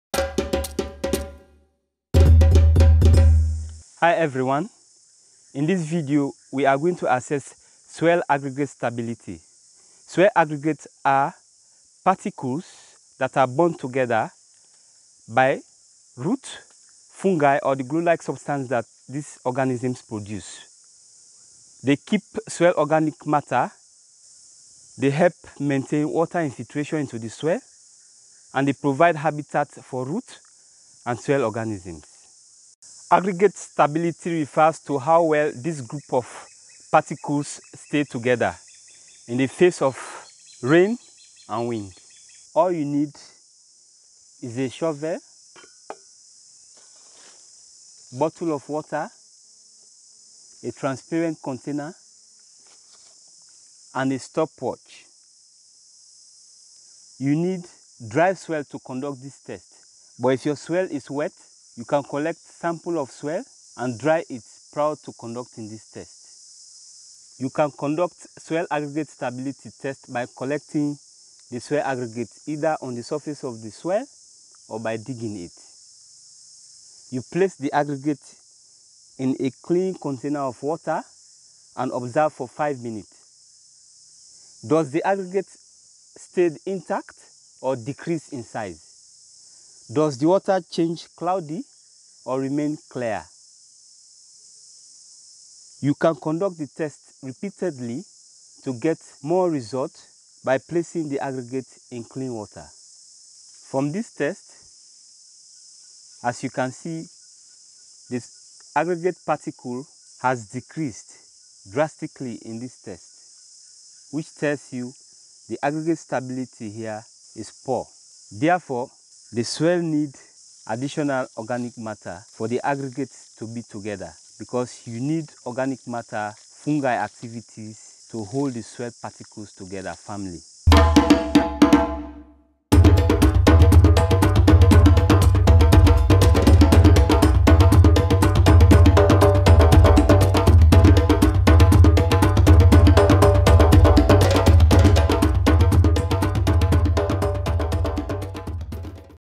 How to Measure Soil Aggregate Stability: Soil Health Tests for Agroforestry Producers
In this series presented by the Regenerative Organic Alliance, you'll learn how to perform simple in-field tests to measure various aspects of soil health. Farmers who apply for Regenerative Organic Certified (regenorganic.org) will need to record and submit their results on a recurring basis. These videos, filmed in Sierra Leone, are specifically meant to help agroforestry producers.

@regenerativeorganic on Instagram

These videos were produced with generous support from Tradin Organic.
Filmed in Kenema District, Sierra Leone. Demonstrations by Mustapha John Bull of Tradin Organic.